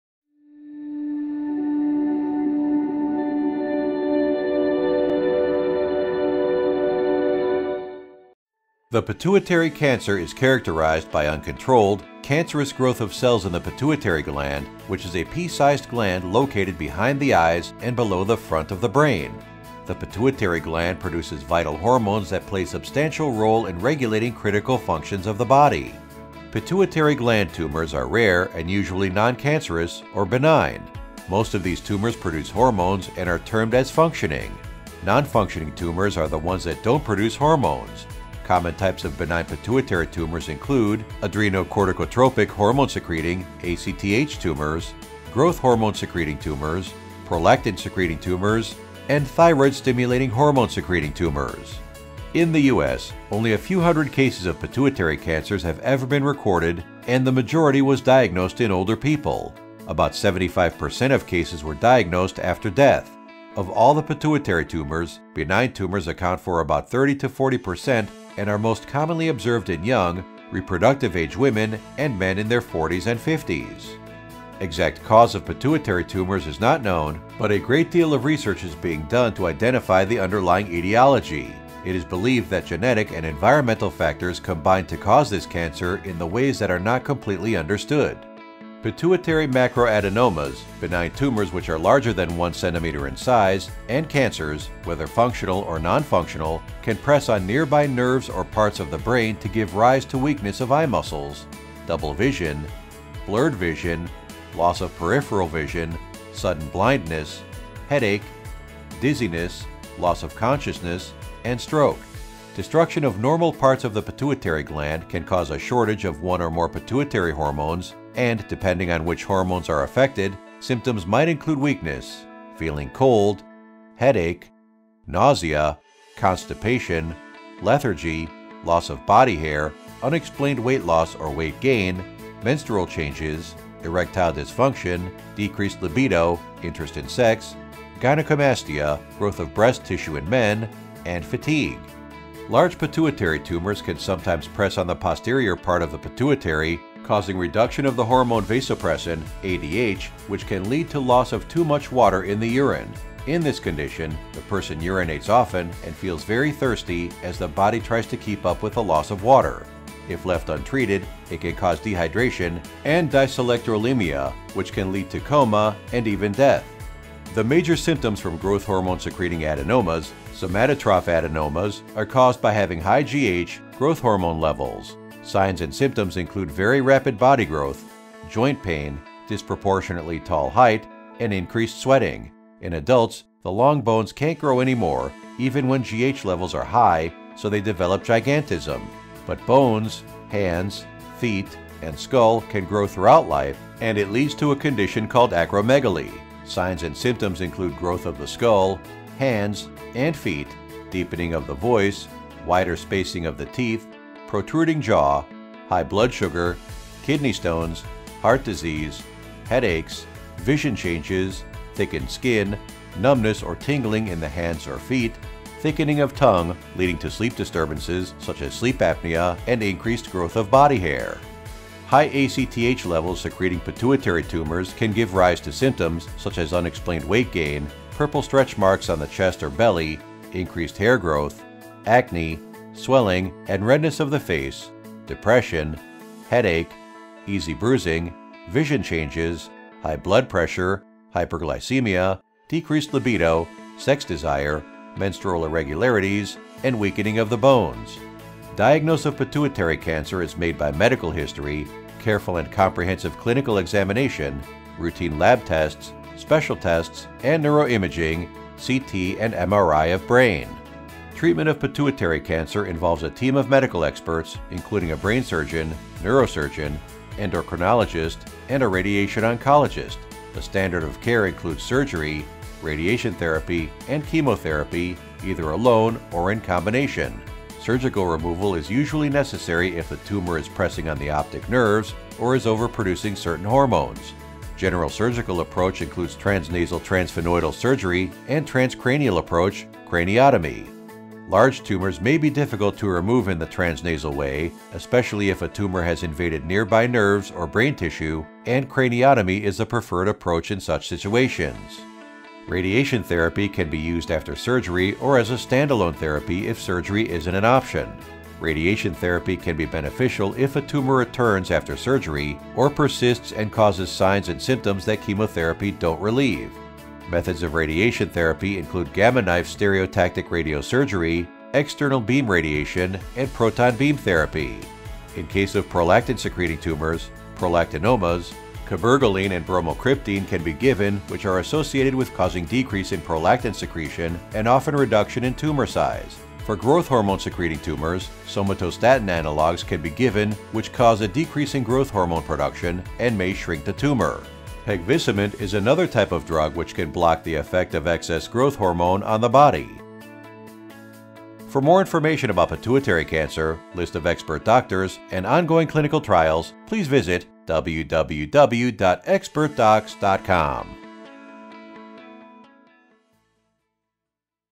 What is Pituitary cancer?
The pituitary cancer is characterized by uncontrolled, cancerous growth of cells in the pituitary gland, which is a pea-size gland located behind the eyes and below the front of the brain.
For more information about list of expert doctors and on going clinical trials please